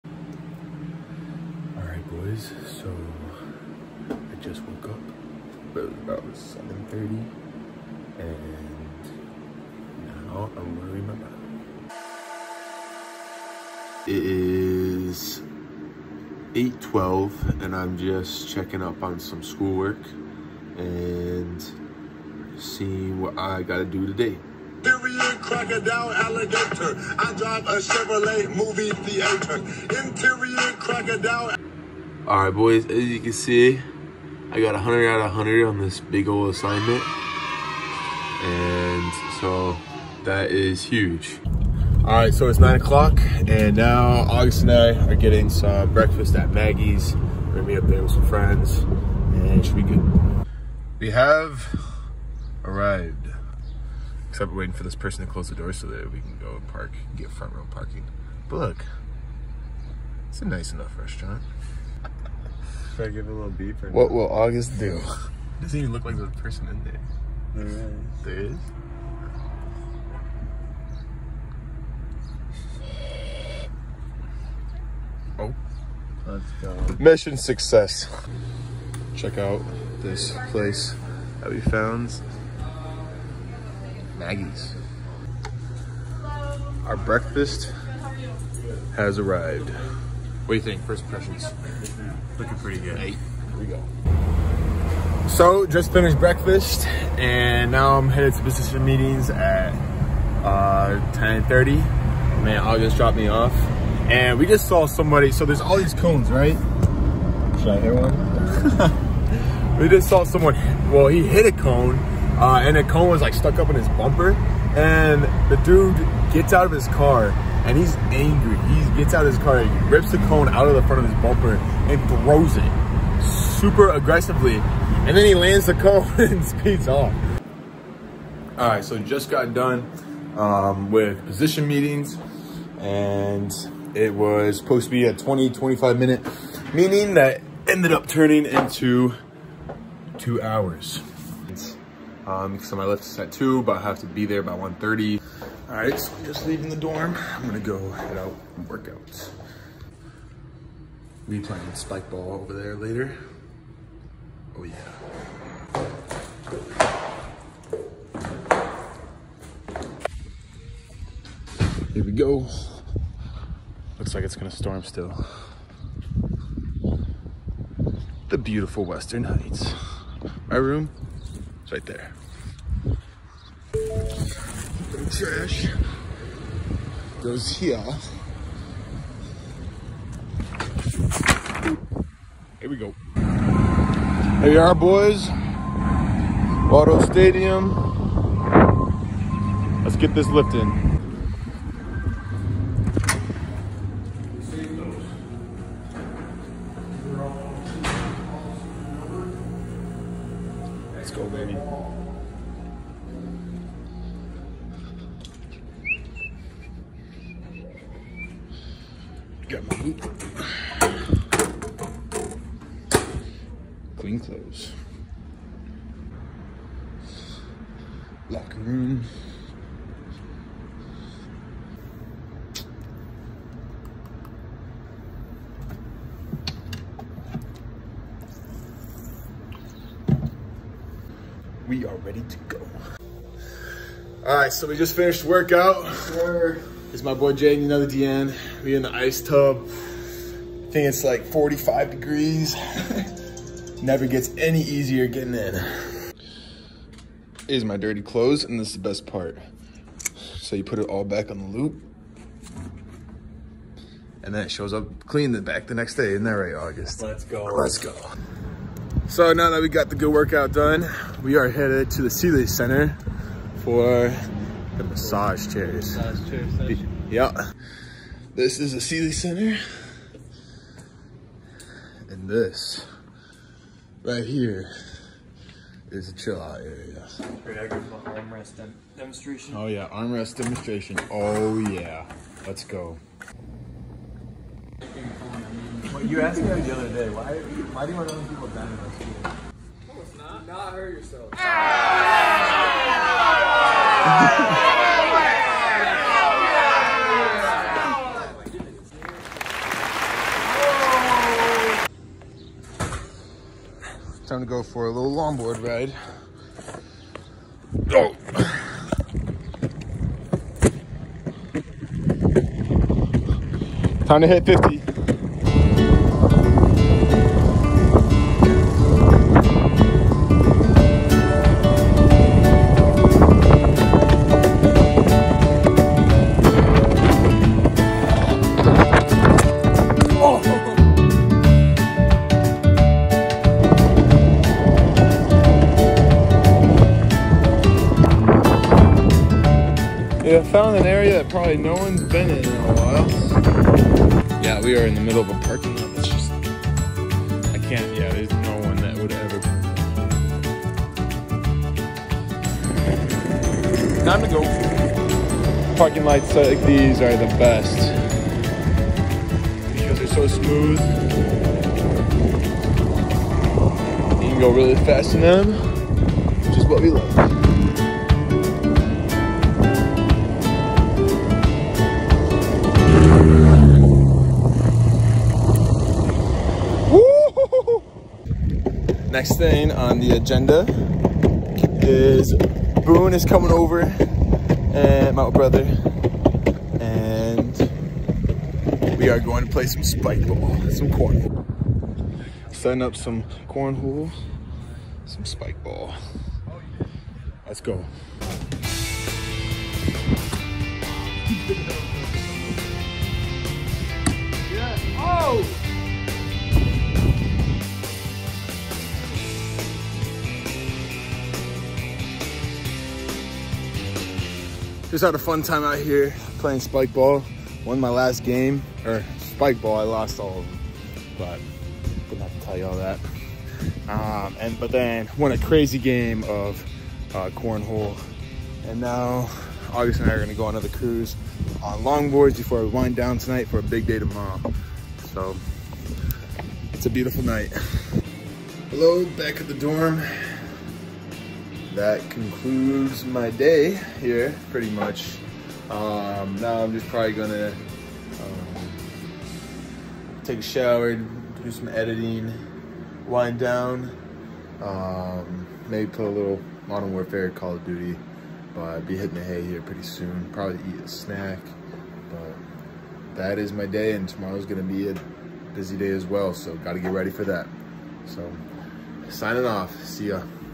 My Day At Western Michigan University (Freshman)
This video is for the boys back home.